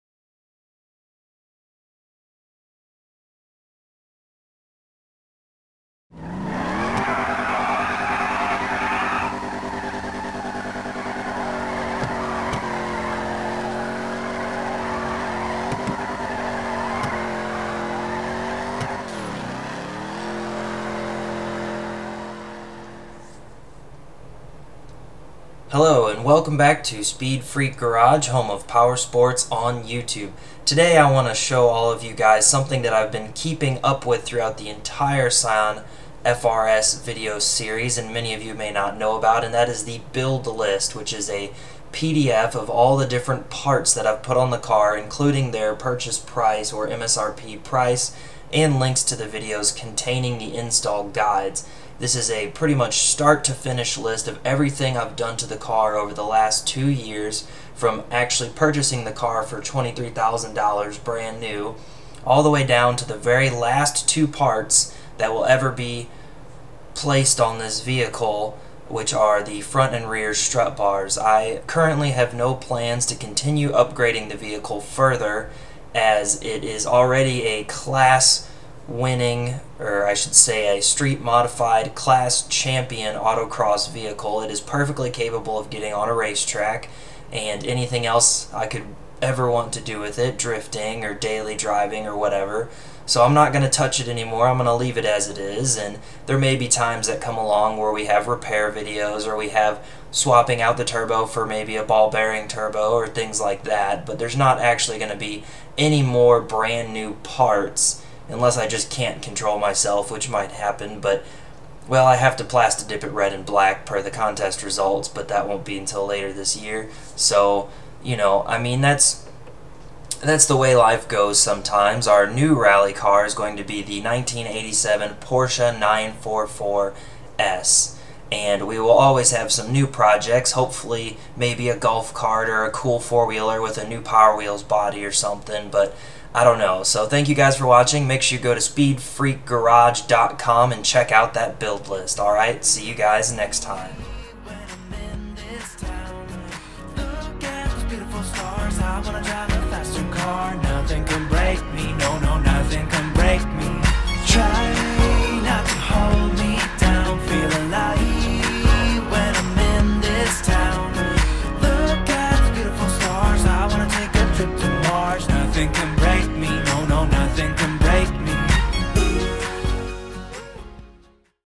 Turbo FR-S Build Log, Racing Parts List & Video Directory
If you are looking for a complete build list of all the custom racing parts I have installed on my custom Turbo FR-S, then go and you should see a build list PDF that contains all the information you could ever want!  A summary of that list is included in the description below.

Perrin Drift Button
GReddy EVOlution GT Exhaust
Varis Carbon Hood
Varis Carbon Trunk
Eibach Sway Bars
Blaupunkt Speakers
Stage 2 Turbo Kit
Drift Armor Drift Bar
Motive Overpipe
Perrin Lightweight Crank Pulley
Perrin Shorty Antenna
Subaru STI PCV Valve
P3 Cars Integrated Vent Gauge
Super Tone Hella Horns
Redline Tuning QuickLIFT PLUS Strut Kit
iJDMtoy 4th Brake Light Kit
Racecomp Tarmac 2 Clubsport Coilovers
Bridgestone Potenza RE-71R
ACT XT Street Clutch Kit
ACT Lightweight Flywheel
Velox Billet Clutch Fork
Velox Clutch Fork Pivot
AEM Front Strut Bar
Cusco Rear Strut Bar

(Scion FR-S)  (Subaru BRZ)  (Toyota 86)